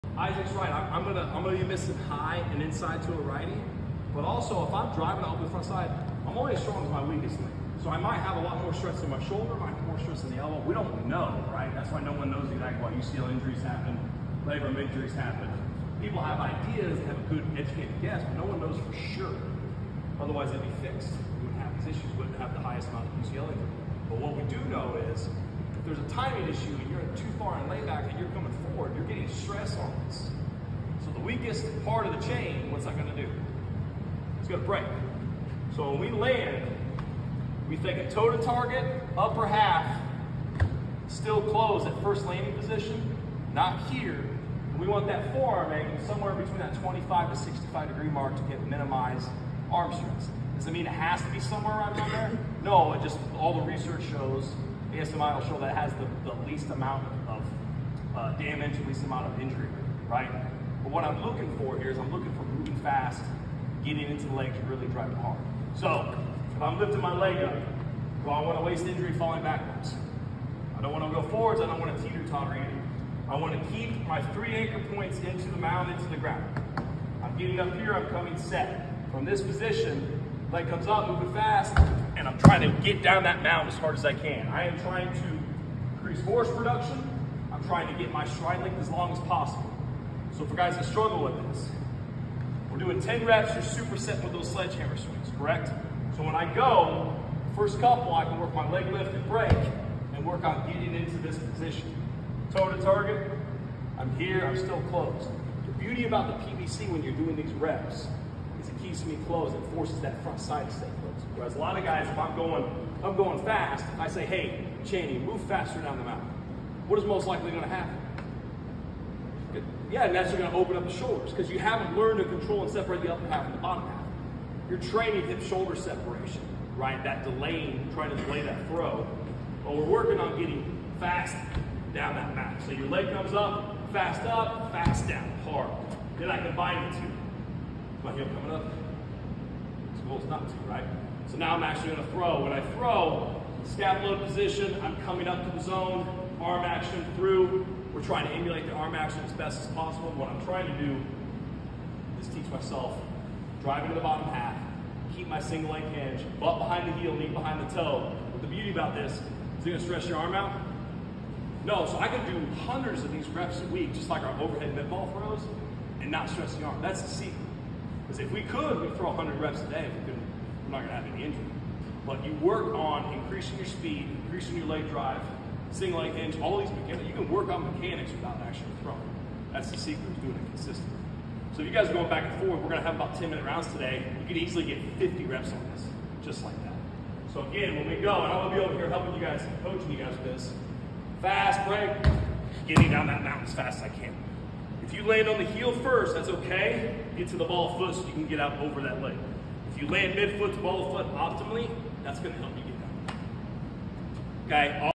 2022 Pitching & Velo Camp Clip | Timing, Injuries, & Low Stress/High Reward Drill
2022 Pitching & Velo Program Educational Clip in our old building! Wow things have changed at LT! Gold clip right here too! A little insight into our orientation day!